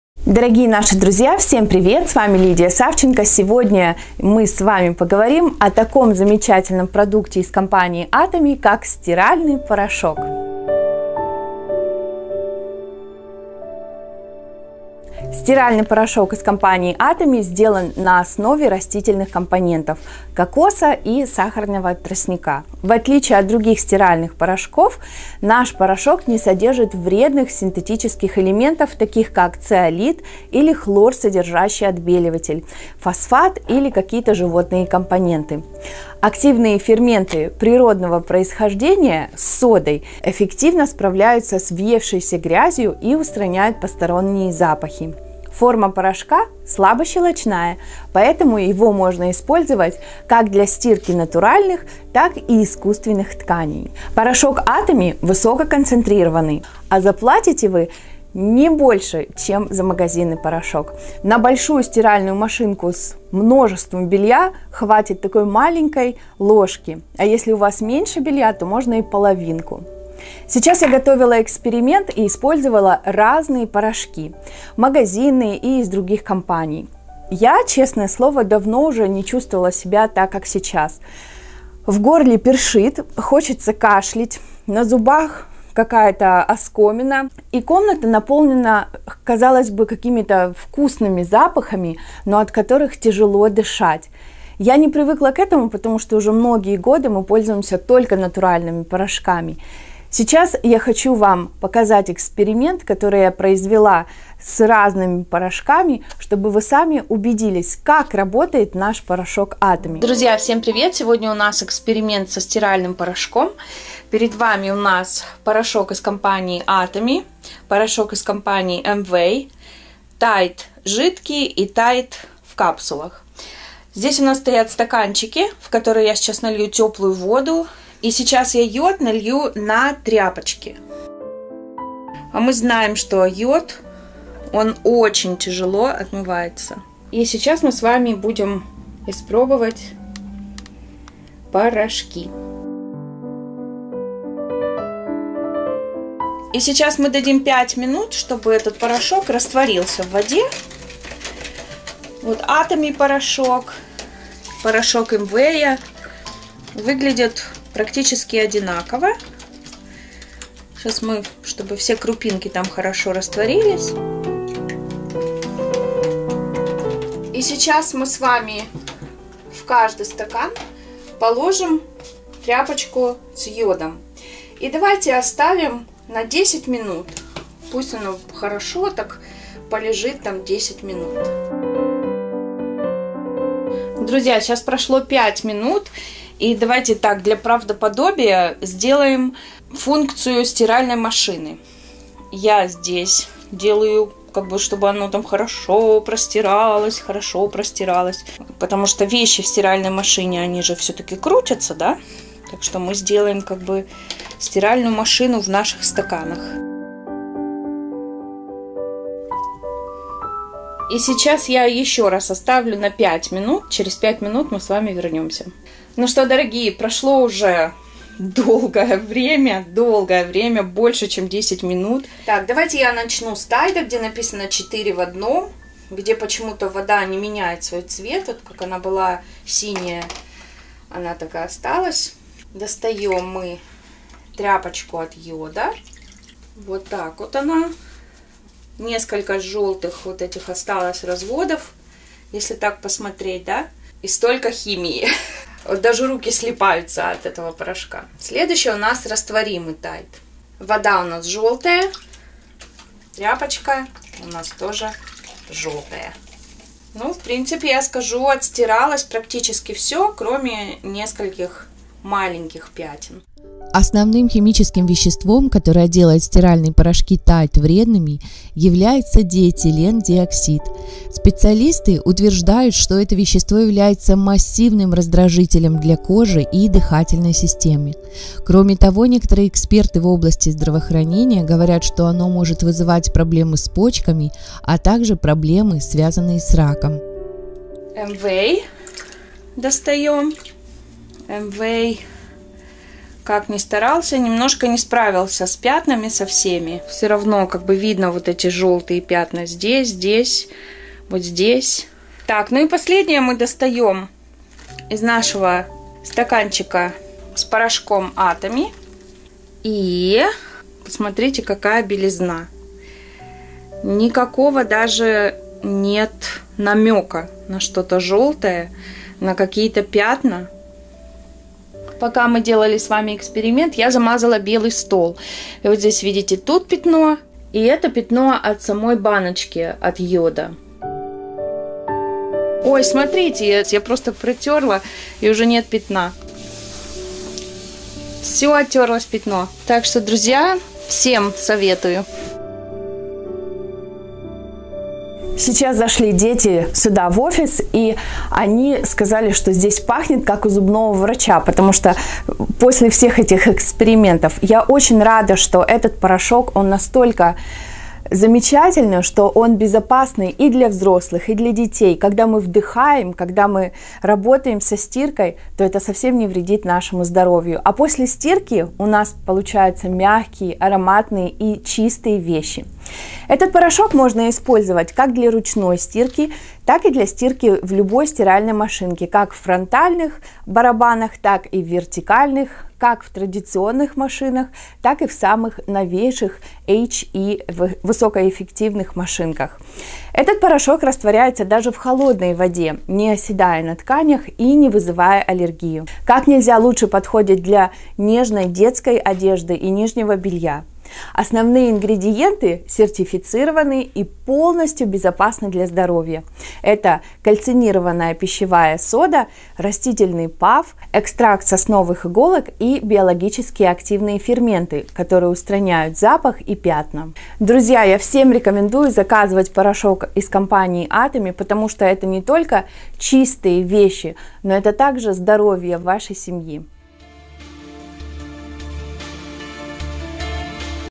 Атоми и Амвей Стиральные порошки
Знаменитый в мире стиральный порошок от Атоми (Корея). Небольшое сравнение стирки с порошками Амвей и Тайд